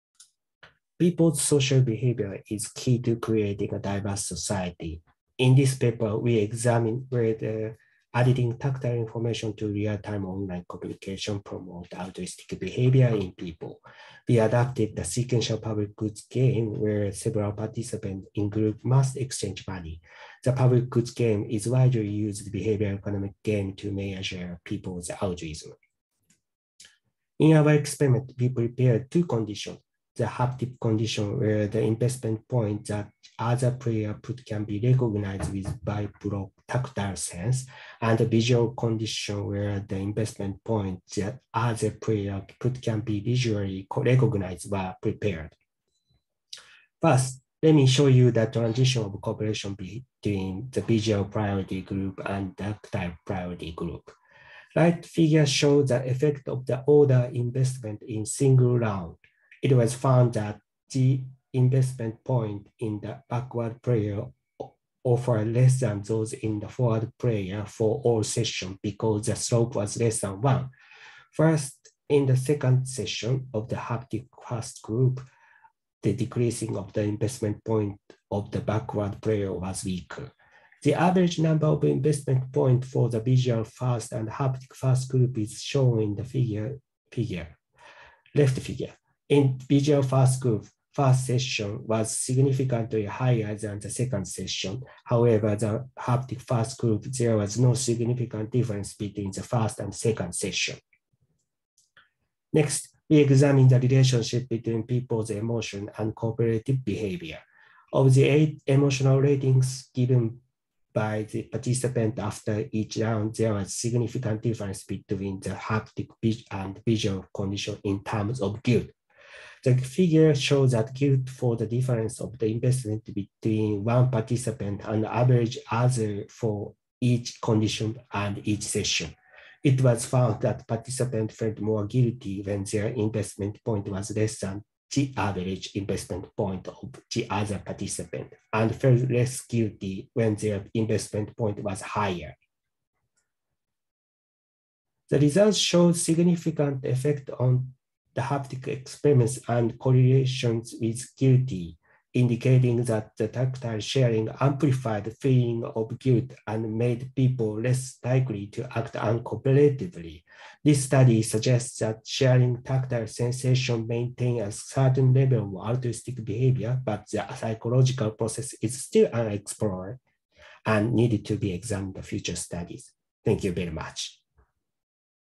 Takeuchi et al., How Haptics Induce Social Behavior, IEEE Haptics Symposium 2022
D. Takeuchi, K. Inukai, M. Suyama, N. Manabe T. Tanichi, Y. Tanaka, J. Watanabe, A. Murata, K. Minamizawa, How Haptics Induce Social Behavior: An Exploratory Study of Public Goods Games with Tactile Sharing on the Internet Using a Behavioral Economics Approach, Proceedings of IEEE Haptics Symposium 2022
Abstract: By integrating haptic presentation technology and certain haptic sensors into online real-time communication, we can acquire subjective information on others through the sense of touch, and use it as a new standard for our own behavior. We believe that this will lead to changes in people's altruistic behavior, including cooperative behavior. This study investigates behavioral changes during tactile sharing using a behavioral economics scheme. We adopted a sequential public goods game where several participants in a group must exchange money. The haptic condition, where the investment points that other player put can be recognized with vibrotactile sense, and the visual condition, where the investment points that other player put can be visually recognized, were prepared. 32 participants were divided into the visual-first and haptic-first groups, and the haptic or visual condition was alternated between the 1st and 2nd sessions. The results showed significant effects on the haptic experience and correlations with guilty, indicating that the tactile sharing amplified feelings of guilt and made people less likely to act uncooperatively.